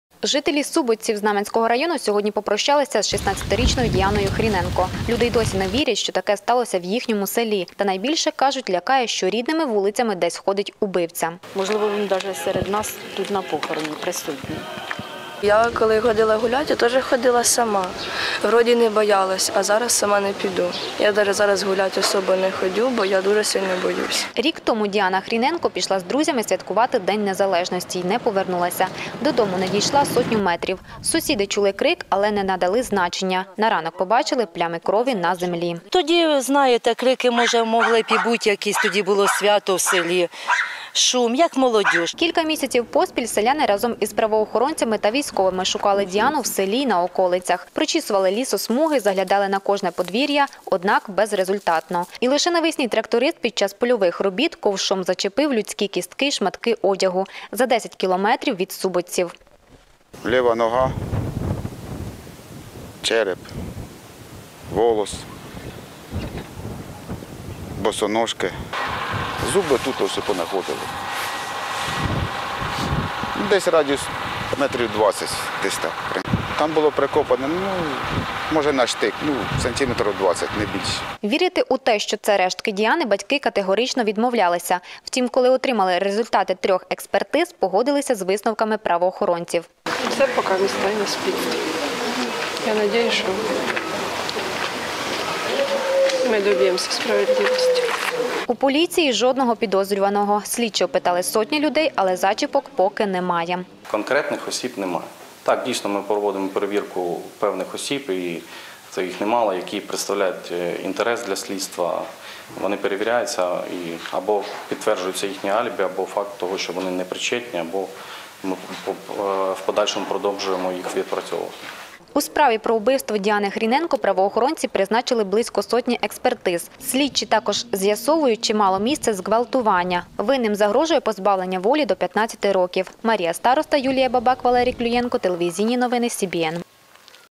Діана Хріненко
Жителі Суботців Знамʼянського району попрощалися з Діаною Хріненко. Люди й досі не вірять, що таке сталося в їхньому селі. Та найбільше, кажуть, лякає, що рідними вулицями десь ходить убивця.